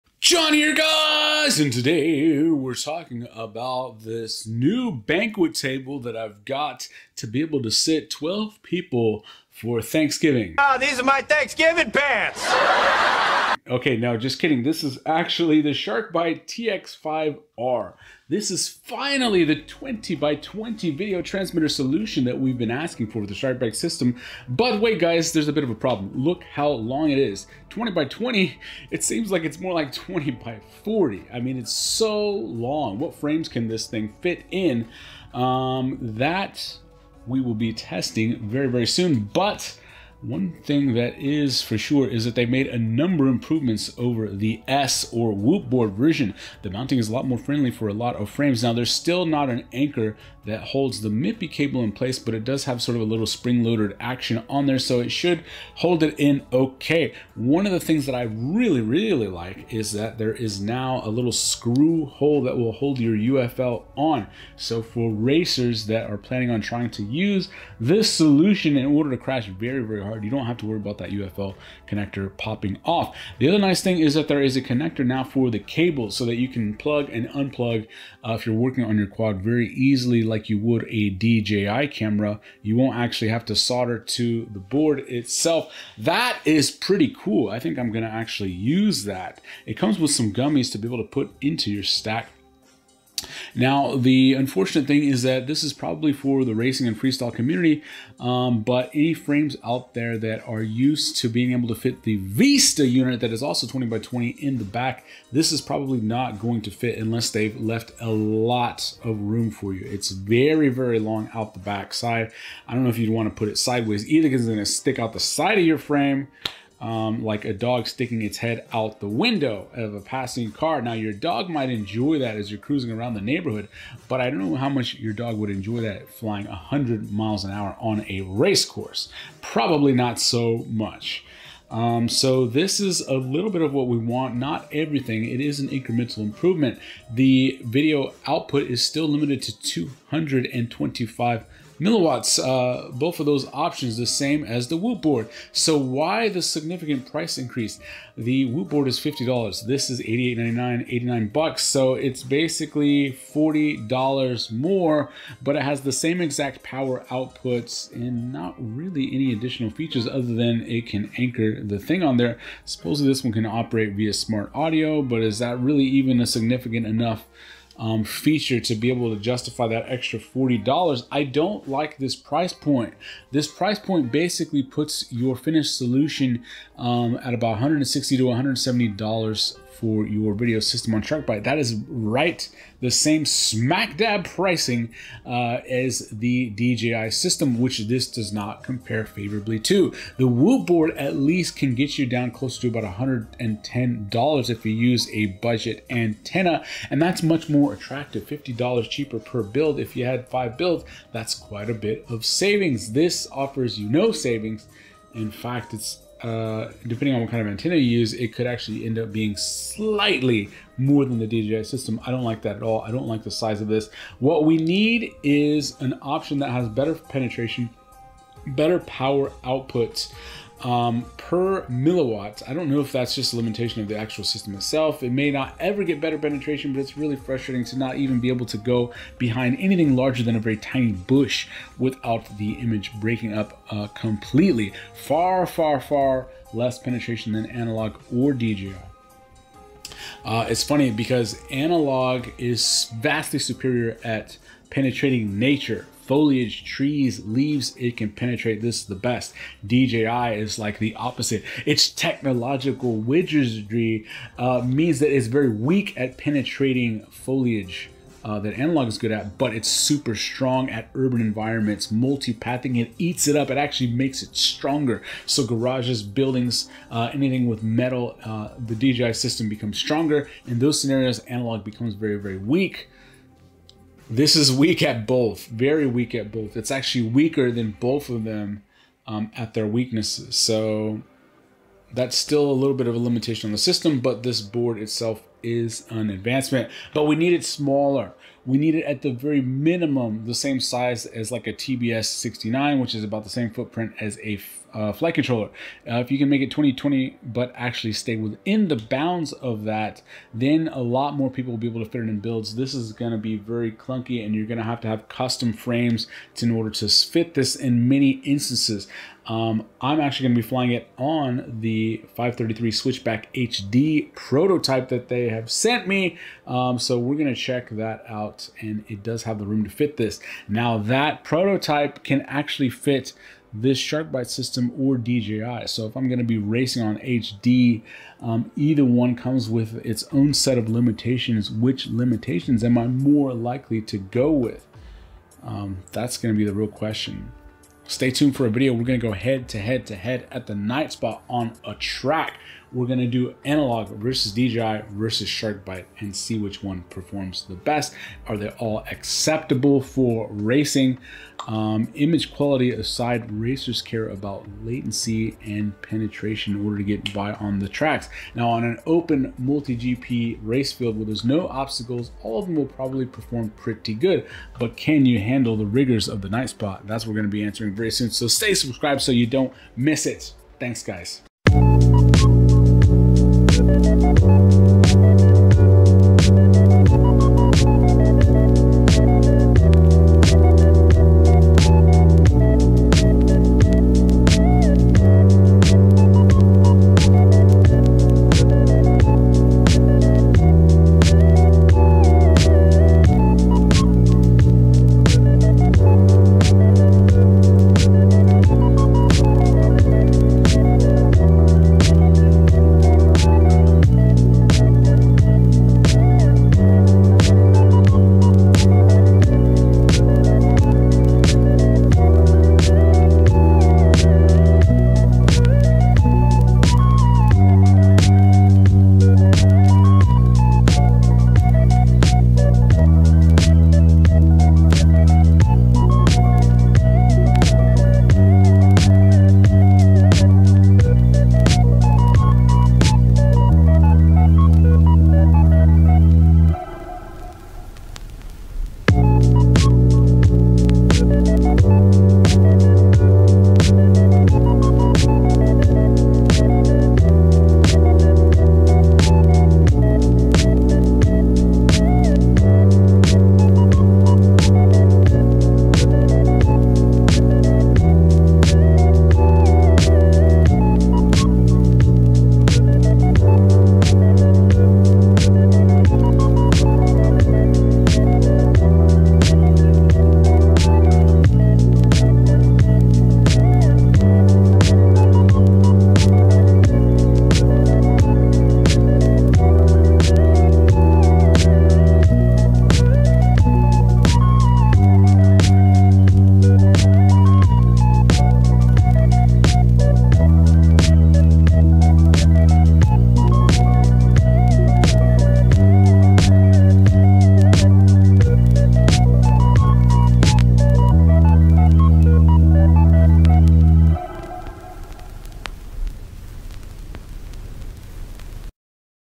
Sharkbyte 20x20 TX5R.1 video transmitter - IT'S HUGE! Can you race with a vtx this large?
Sharkbyte 20x20 TX5R.1 video transmitter